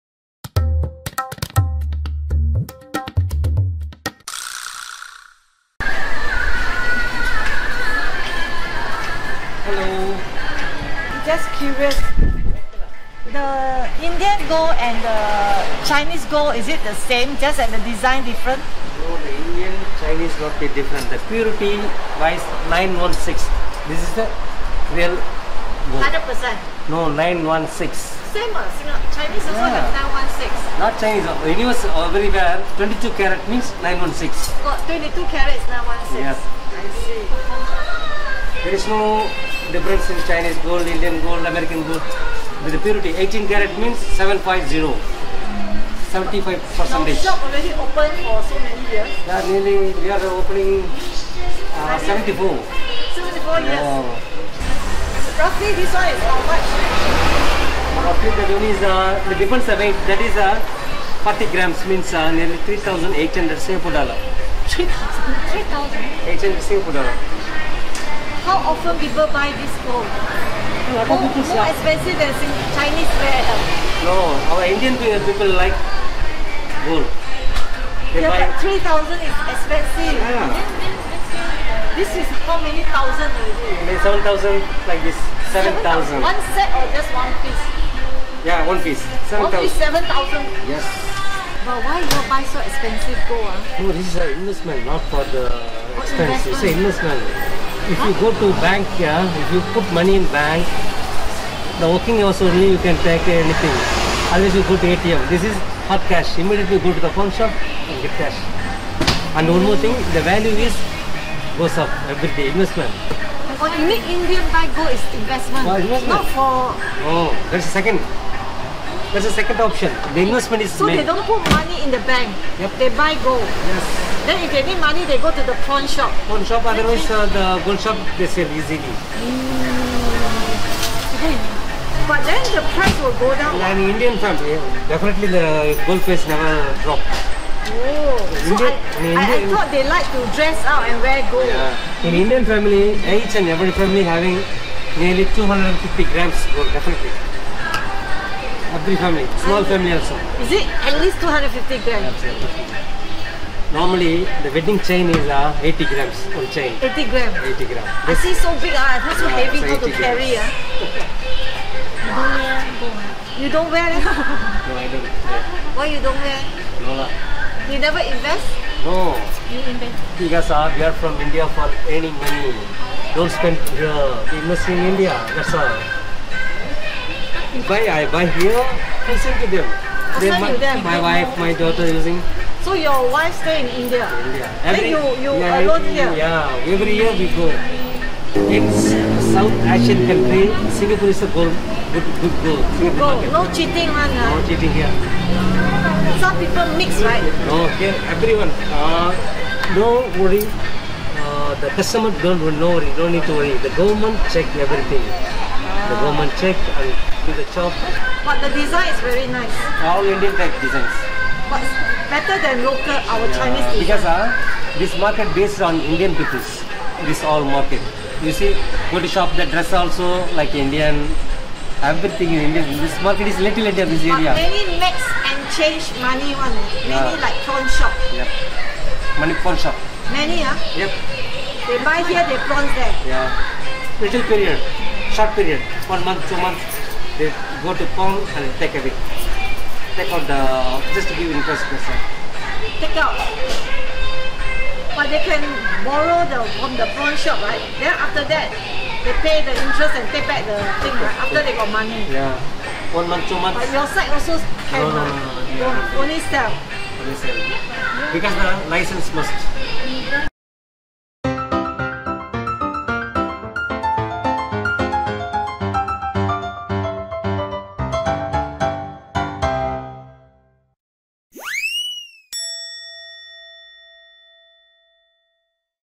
【シンガポール】リトルインディアの宝飾店に潜入！インド人は金がお好き？リトルインディアに多い宝飾店と質屋の謎が明らかに！安心安全！シンガポール政府お墨付きの純金！916は22カラットの黄金！？
リトルインディアを散歩するとゴールドの腕輪やネックレスを売る宝飾店が多いことに気づくだろう！そして質屋も多い！その謎が今回明らかとなりました！

インド人の多くは金の腕輪や指輪、女性なら金のネックレスでオシャレしているのをよく見ます！ところがこれ！ファッションだけじゃなくて投資（現金の代わり）として買うそうなんです！

インド人は銀行などを信用していないということでしょうか！？
おじさんによるとインド人の１家族あたり250グラムの金は必ず所有しているとのこと！